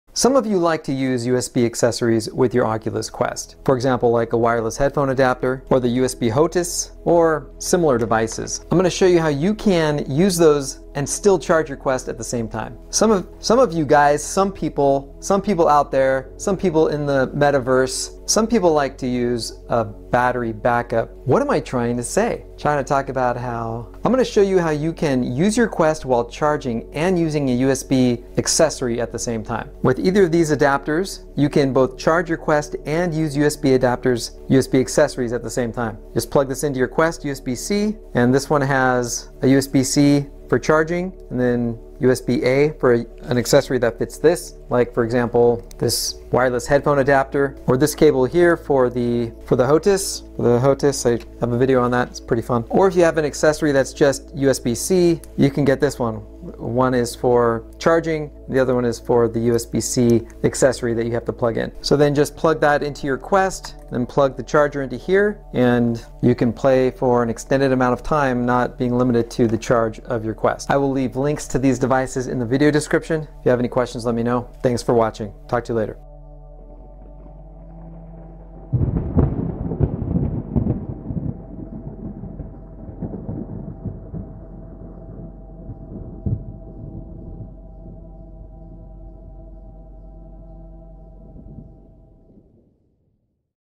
CHARGE *AND* ACCESSORIZE the Quest at the SAME TIME with this USB-C Y-Splitter Adapter
Black Adadpter
Silver Adapter

Don't have a Quest 2?

In this video I show you that you can use this adapter to both CHARGE your Quest and use USB accessories at the same time.